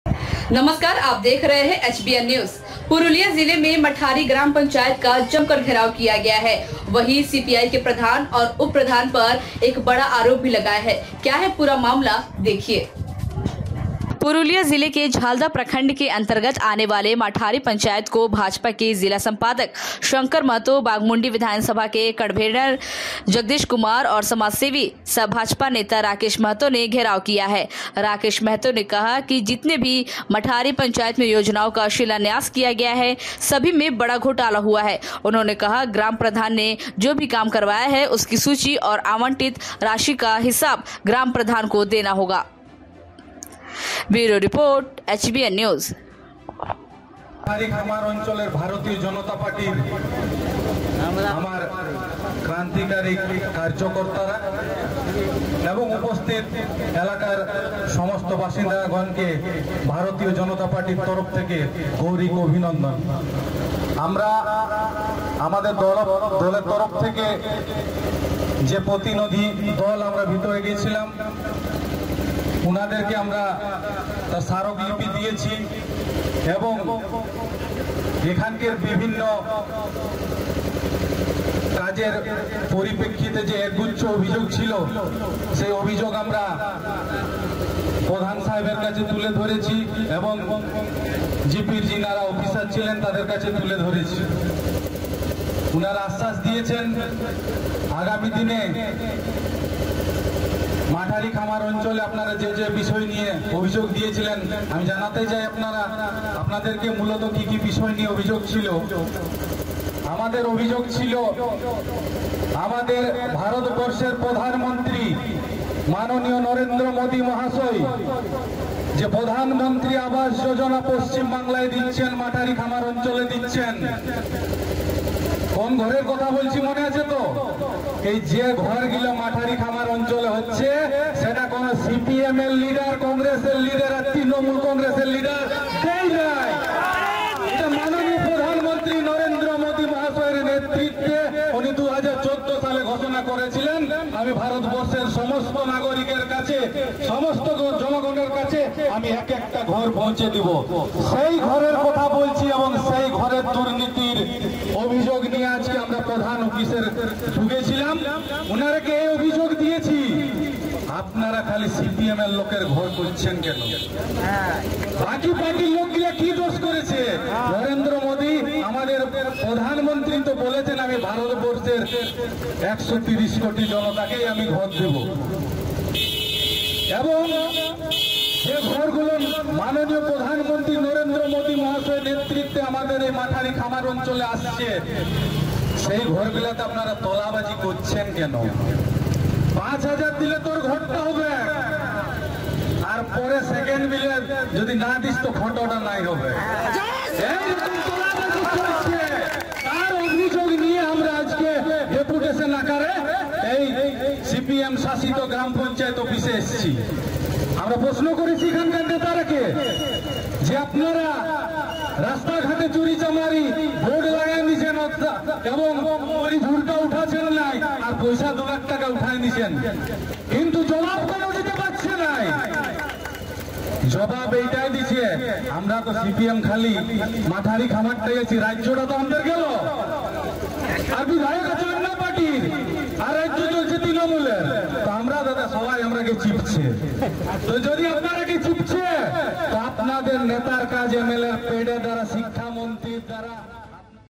CPI के प्रधान और उपप्रधान पर लगाया बड़ा आरोप, जानिए क्या आरोप लगाया? | HBN News
CPI के प्रधान गंगा सिंह मुरा और उपप्रधान हेमंत महातो पर लगाया बड़ा आरोप, जानिए क्या आरोप लगाया? | HBN News | Jo Sach vhi Khabar